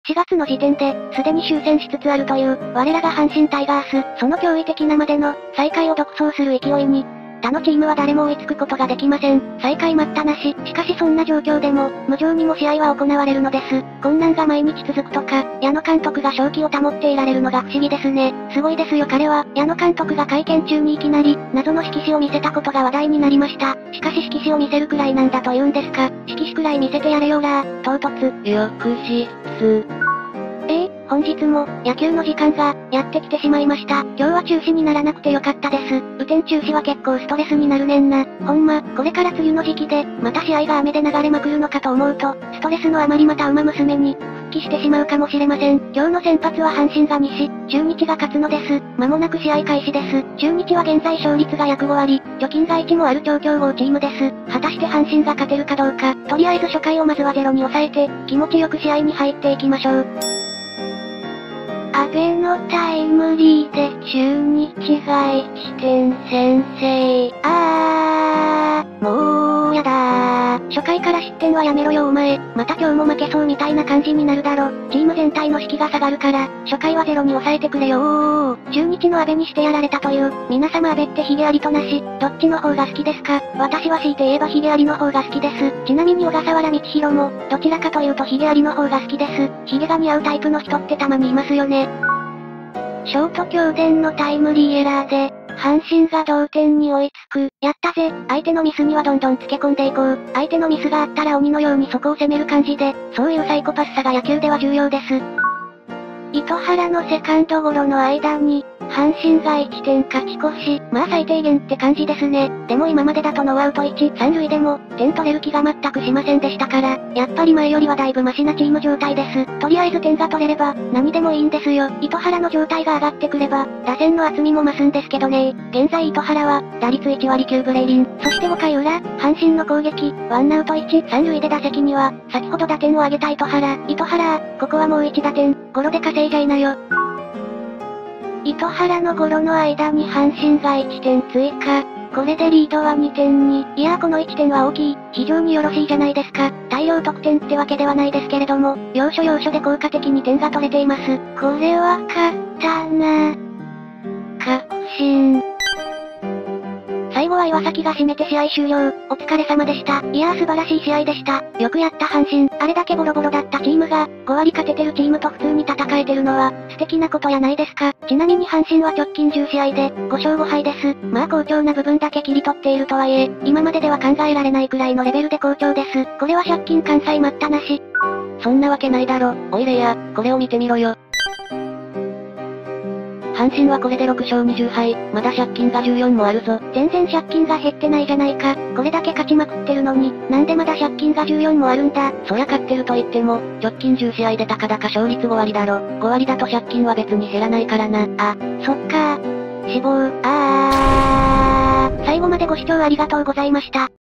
阪神が勝ってはしゃぐ男の娘【プロ野球試合結果】阪神タイガース－中日ドラゴンズ
死球に関しては本当に申し訳ない思いです。　　いい試合だった……これからもこれを続けてください。　／こんにちは、赤木レイアです。気軽に見られる３分動画を中心に投稿してます。